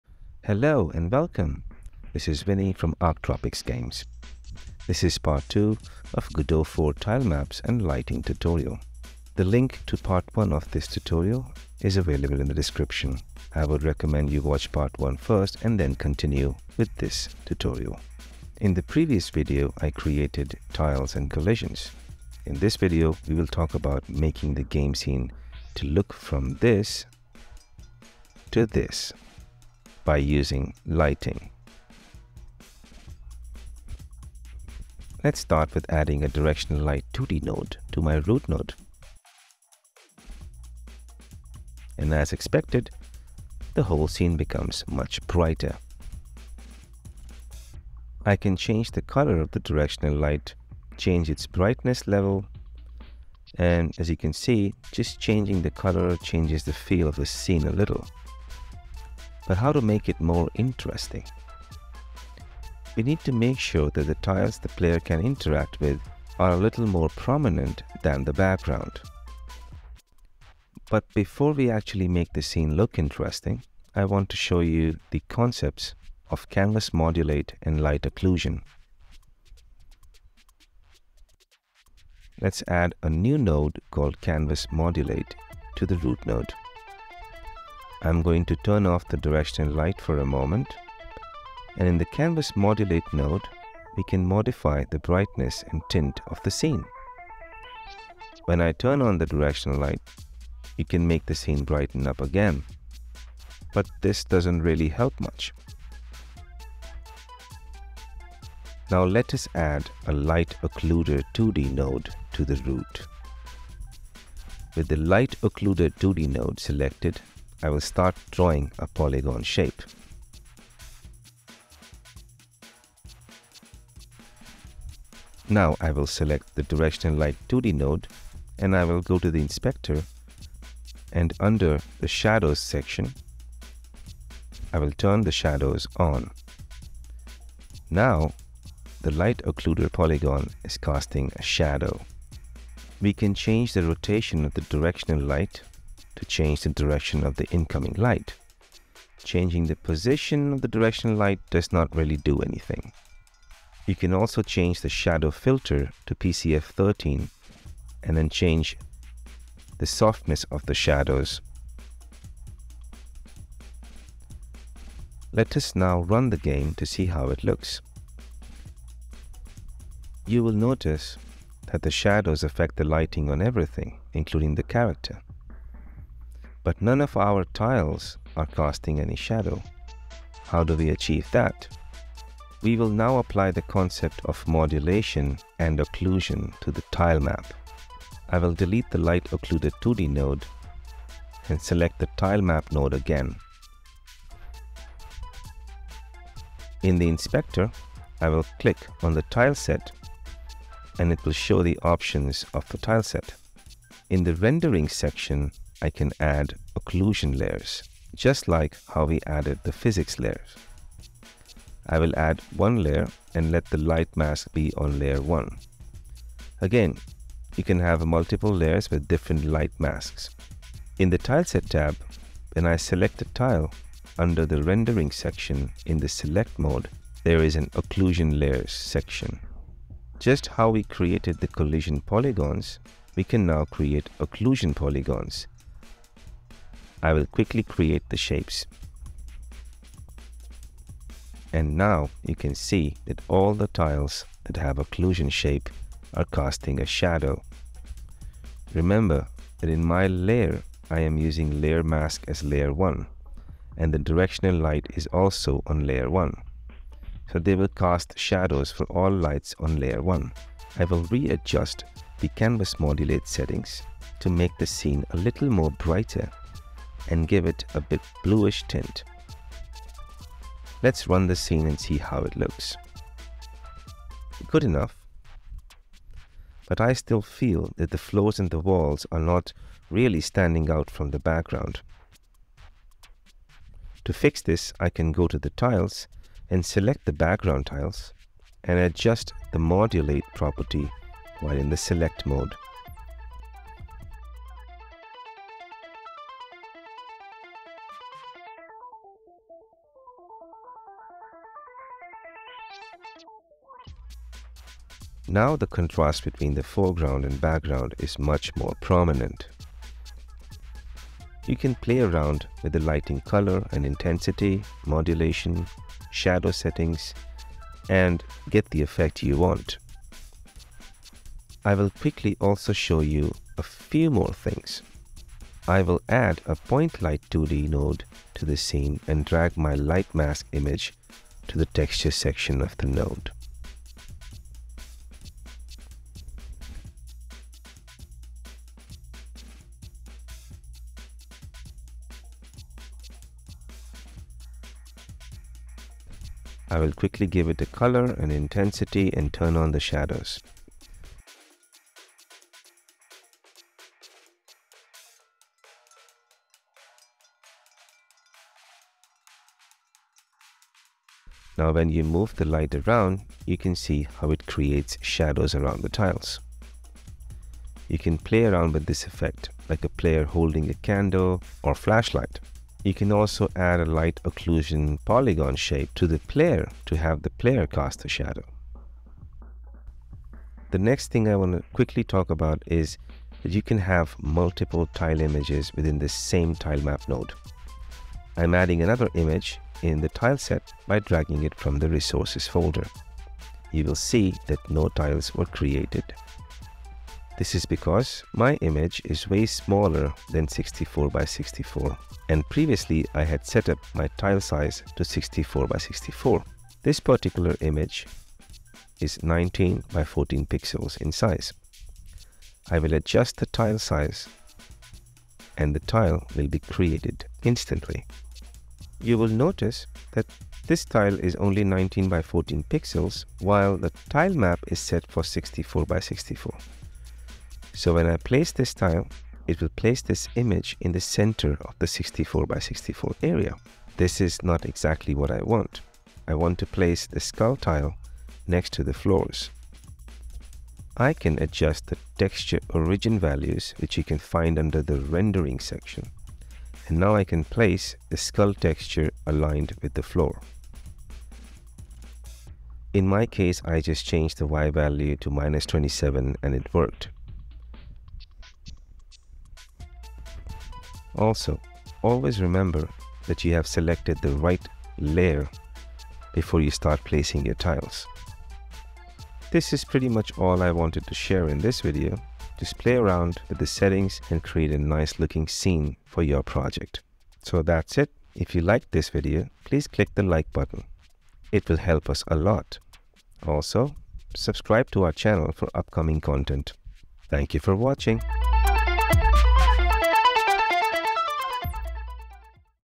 Godot 4 Tutorial: Tilemaps and Lighting - Part 2
This is Part 2 of the "Godot 4 TileMaps and Lighting for a 2D game" Tutorial! In this video, I will show you how to create beautiful levels using the new 2D Lighting in Godot 4 Engine, You will learn how to create Directional Light, Shadows and apply Light Occlusion and Modulation to Tiles

By the end of this tutorial, you will hopefully have a basic understanding of how to use Godot 4 Lighting for a 2D game.

Godot Version Used: Godot v4.0.2

You can download these game assets for free from Itch.io

00:00 - Introduction
00:34 - Directional Lighting
01:10 - Modulation and Occlusion
02:05 - Enabling Shadows
03:10 - Creating Light Occlusion in a Tile
04:35 - Adjust Modulation in a Tile
05:25 - Creating a Point Light in 2D
06:20 - Using Multiple Images in the TIle Map
07:48 - Outro